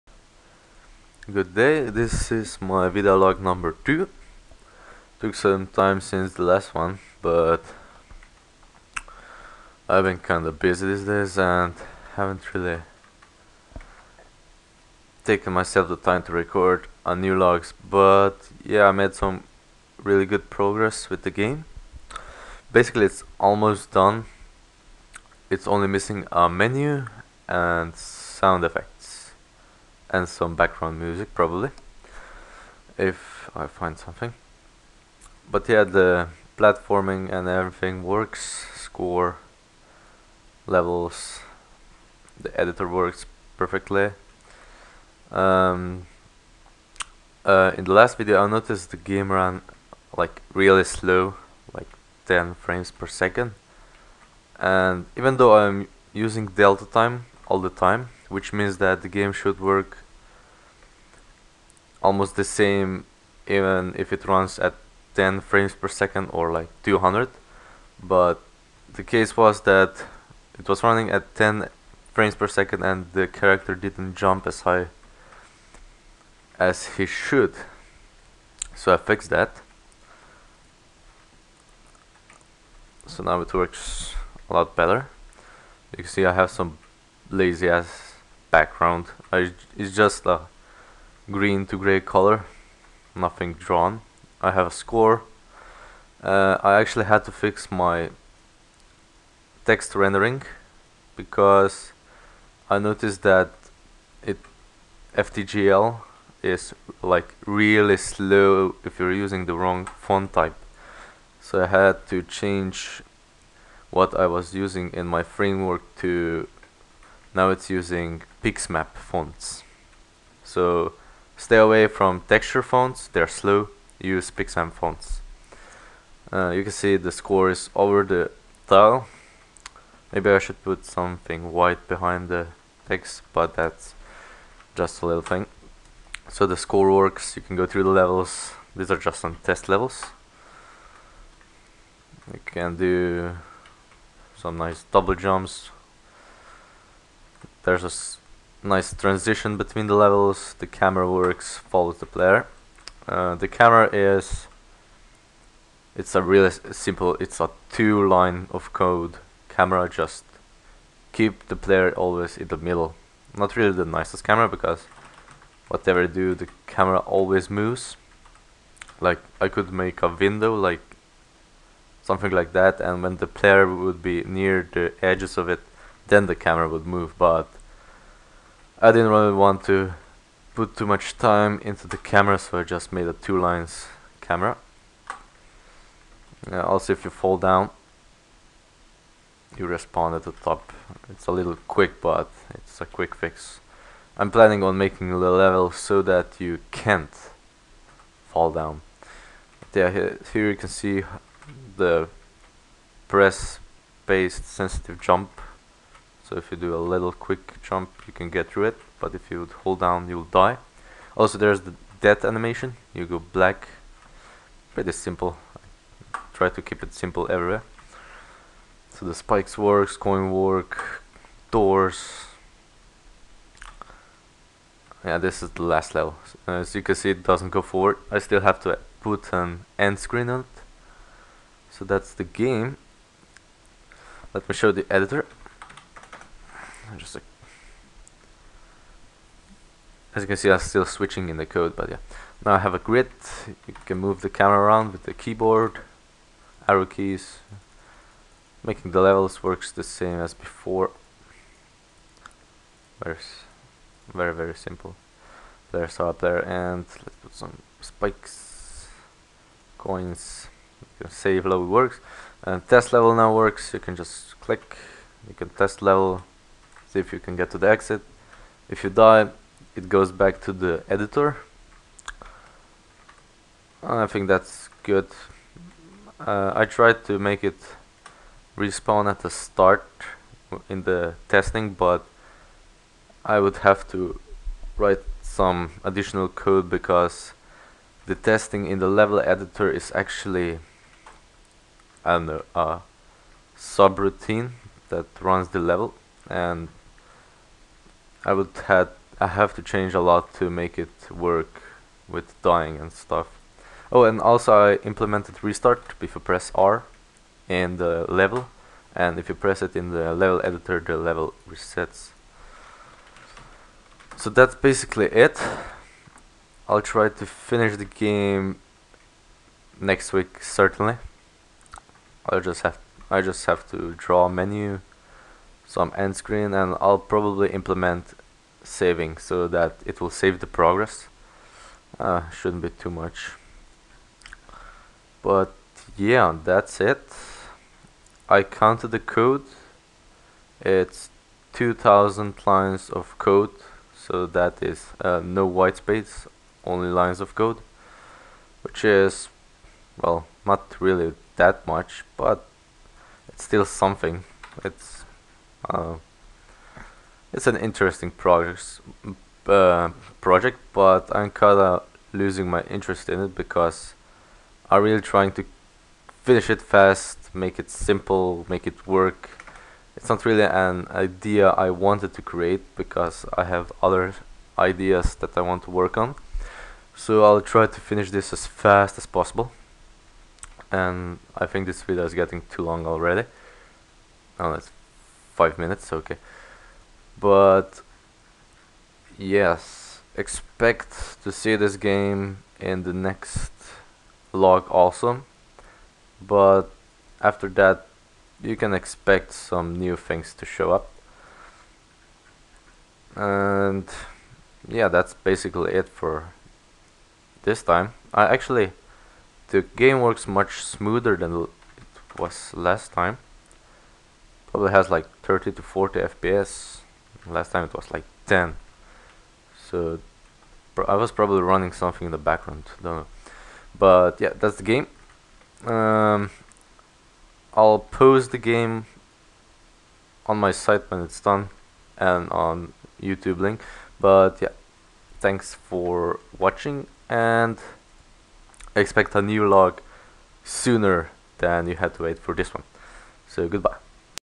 Red's video log #2
Just showing the current school project. Almost finished but still needs some small things. You can see most of normal platforming things implemented.